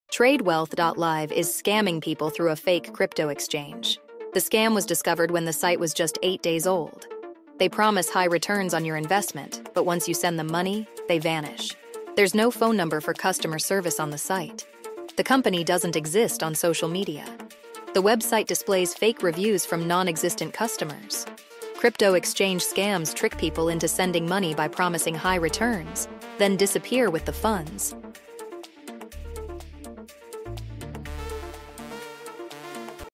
Tradewealth.live SCAM
Beware of Tradewealth.live! This fake crypto exchange promises high returns but vanishes with your money. No customer service or social media presence. Fake reviews. Stay safe!